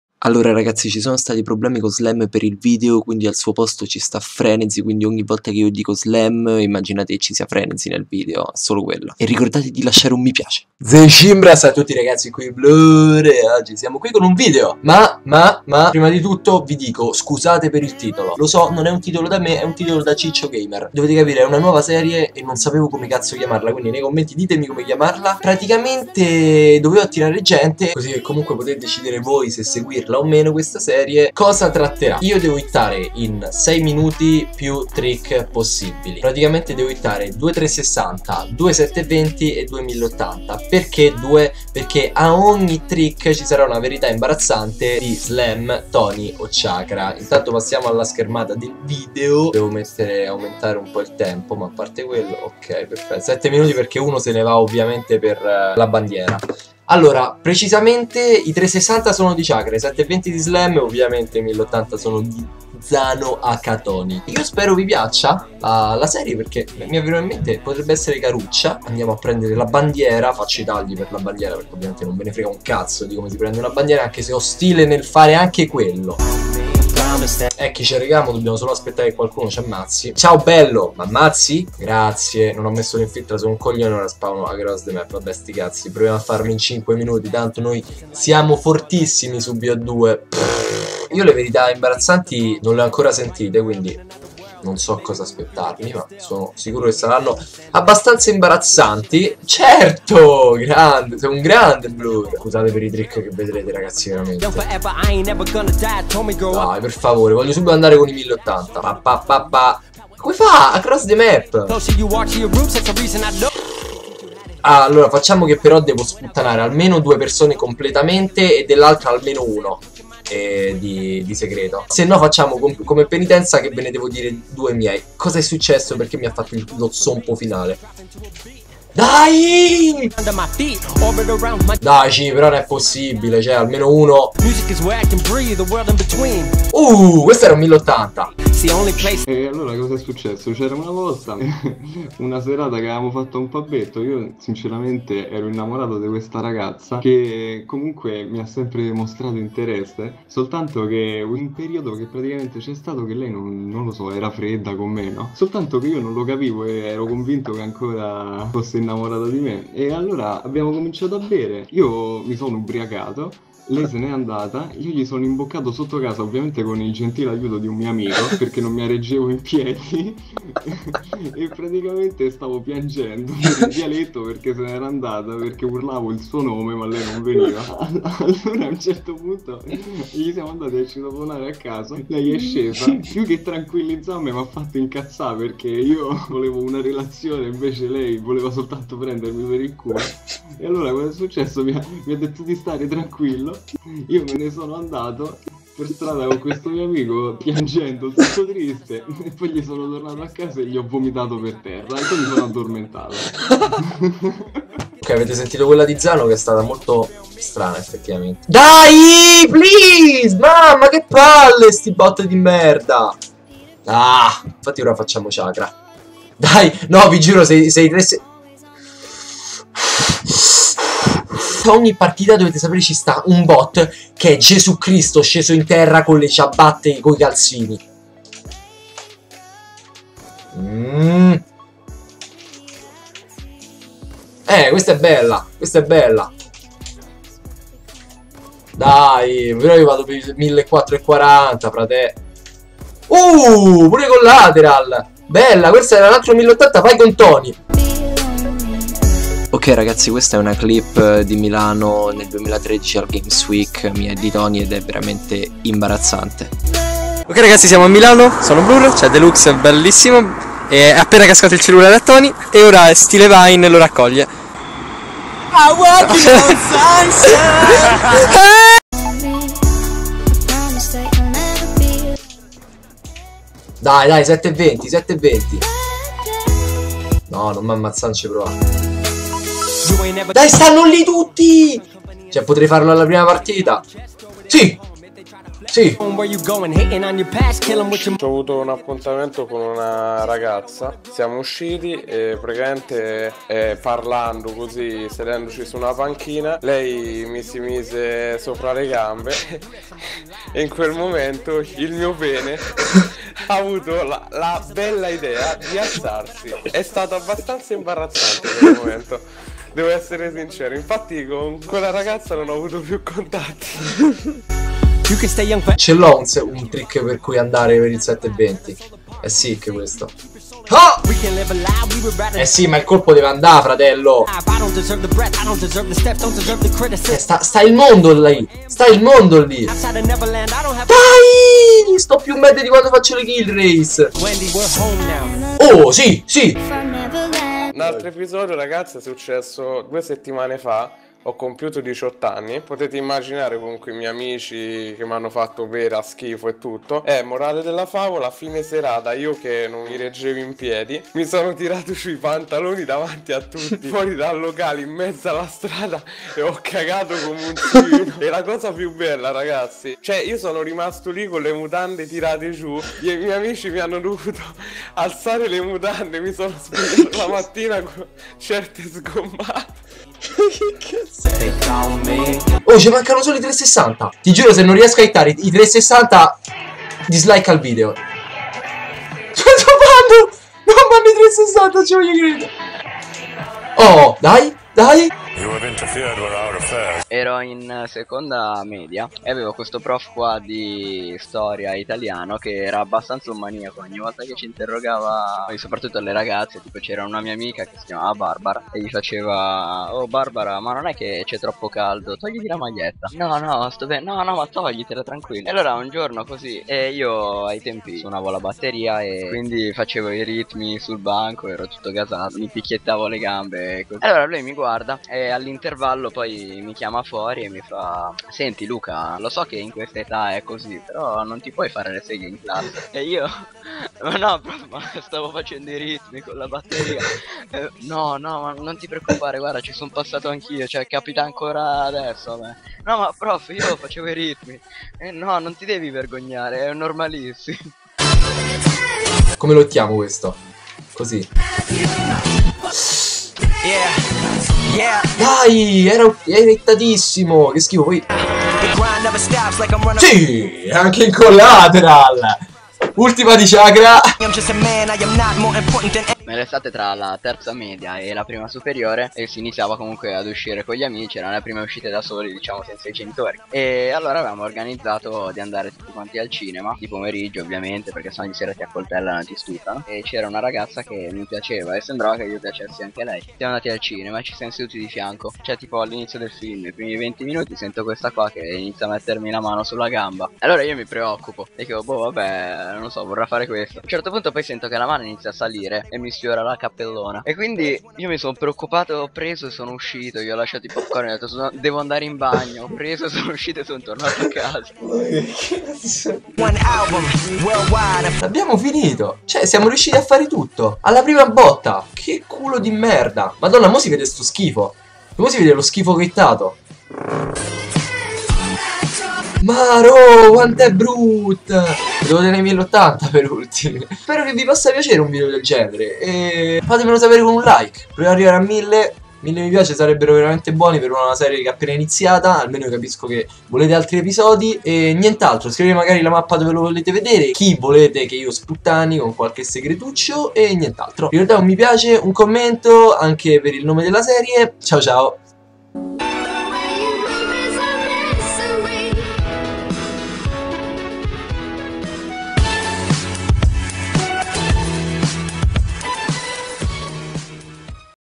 OGNI TRICKSHOT UNA VERITA' IMBARAZZANTE!! - ft.Tony,Chakra,Frenezy!
1OOO MI PIACE PER IL SECONDO EPISODIO!!
Ho anche un canale di Streaming! ツ